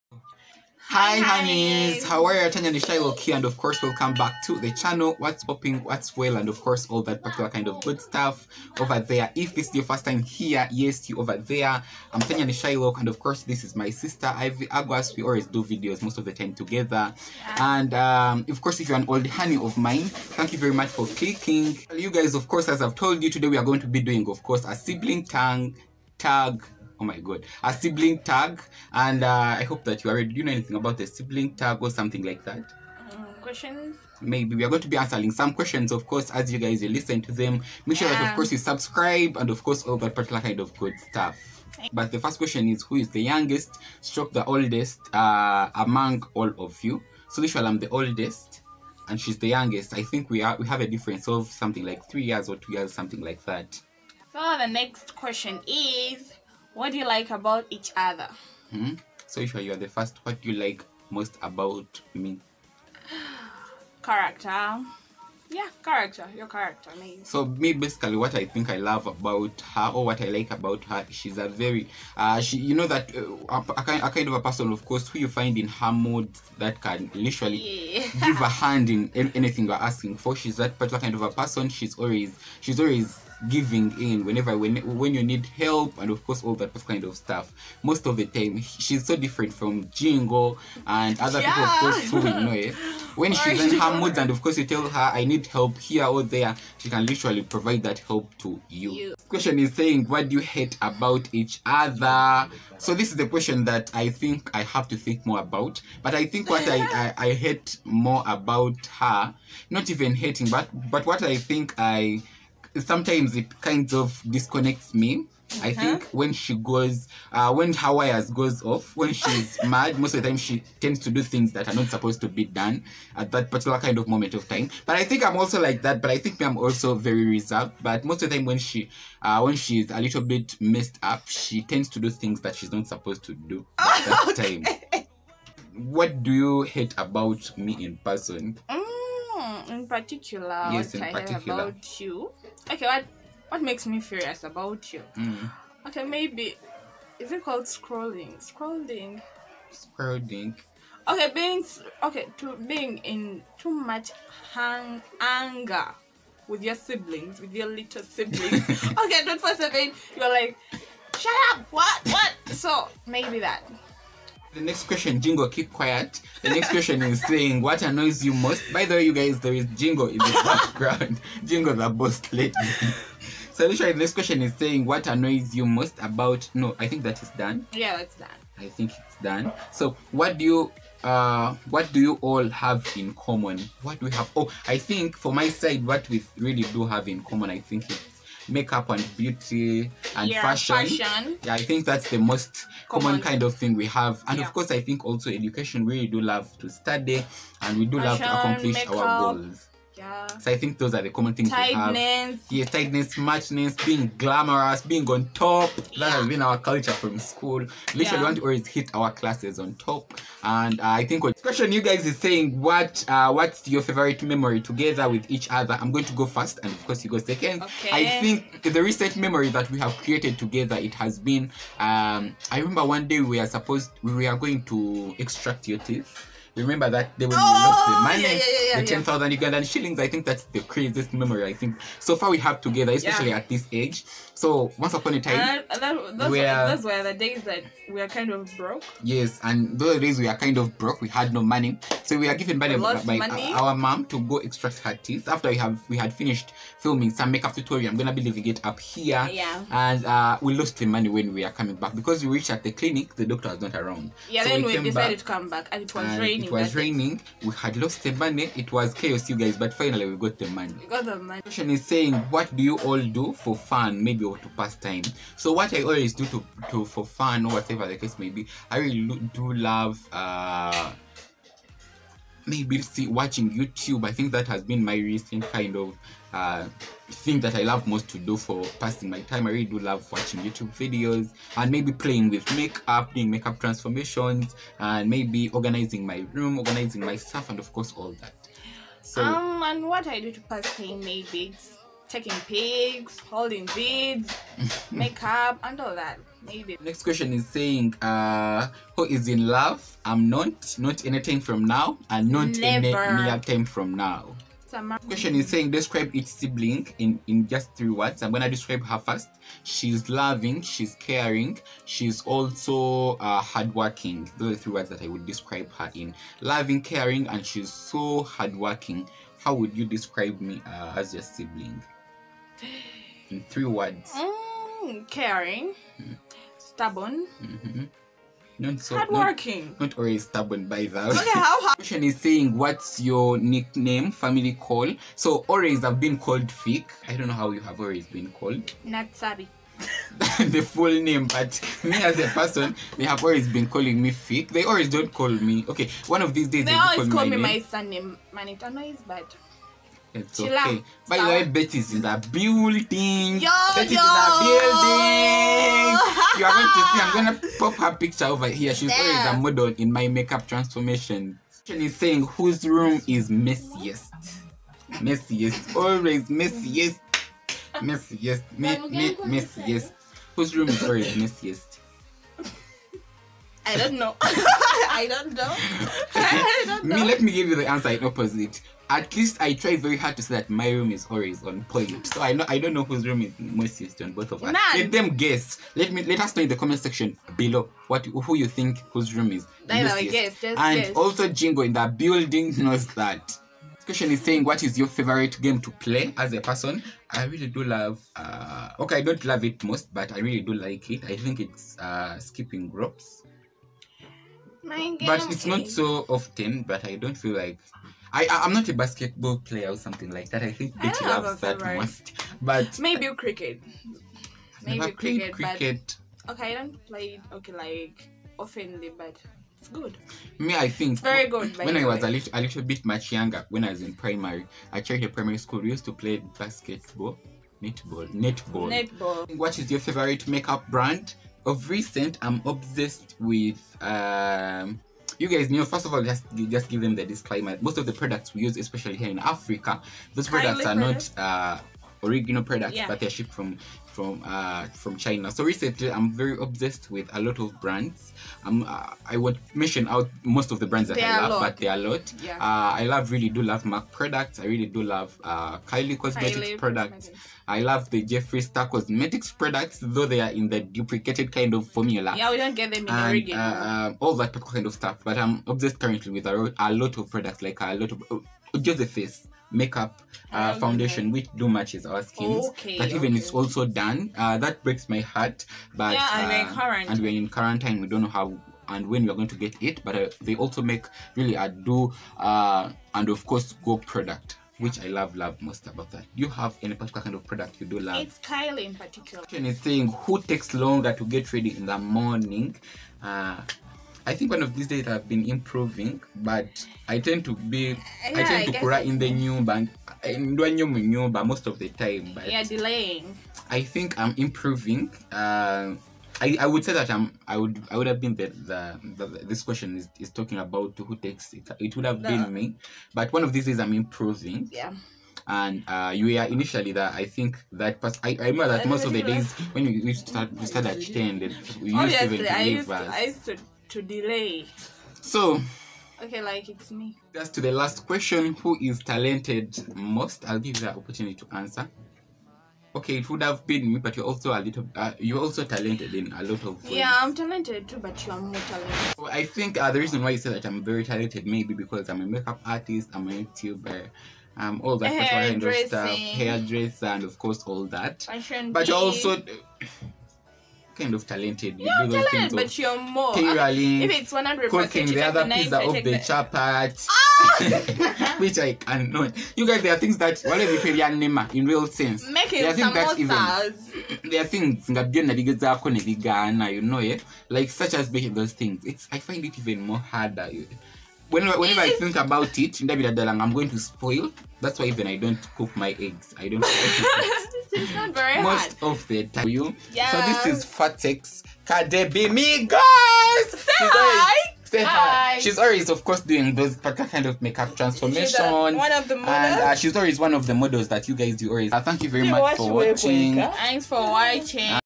Emotional😭 SIBLING TAG! It has been a journey(ft my sister ivy)//Tanya nishailoc
Our emotional sibling tag. So i love my sister alot and we have grown together to be inseparable and we do care for and take responsibility of each other, so in today's video we take about our life and the history we have made together for over all these years.
We also do answer questions related to our past and present❤

Most asked question➡
Age;21
Career;makeup artist and nurse

Siblings tag, sibligtag,emotional sibling tag